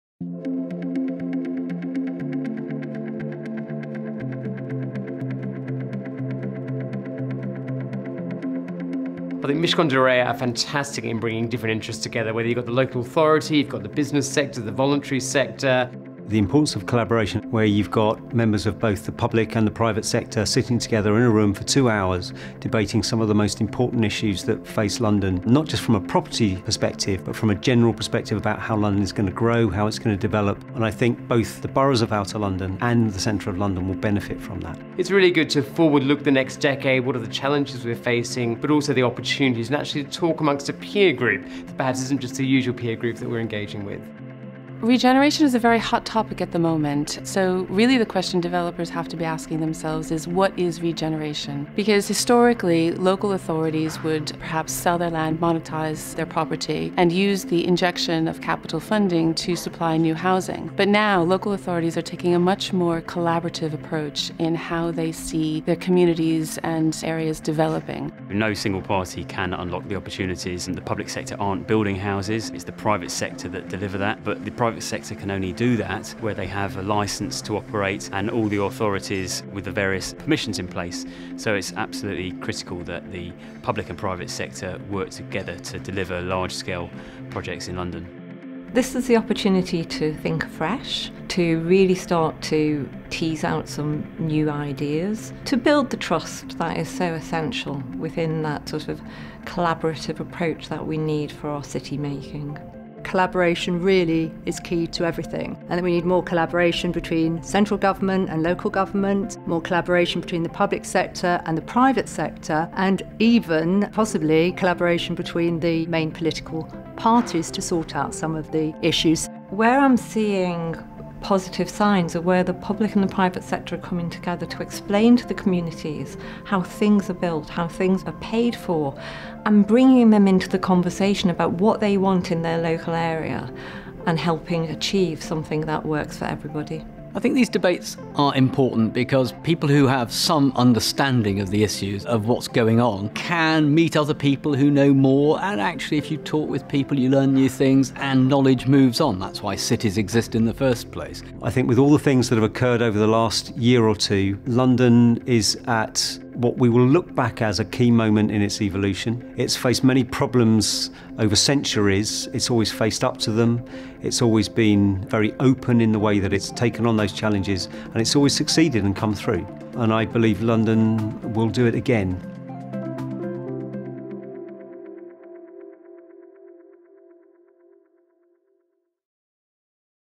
London post… everything
The Real Estate department recently hosted a round table debate with Patricia Brown of Central as part of our Collaboratory series. The theme, 'London post… everything!', enabled our guests, drawn from the senior ranks of developers, local authorities and thought leaders, to examine the many issues facing London at this inflection point. The areas we explored ranged from Brexit, the Grenfell disaster, the housing crisis, local authority funding for housing and the public realm, to the urgent need to innovate. For all these critical issues facing London, the debate proved to be a positive and useful opportunity for open discussion. We interviewed some of our guests before and after the debate - the short film above features comments from those listed below, capturing the essence of the issues that were raised.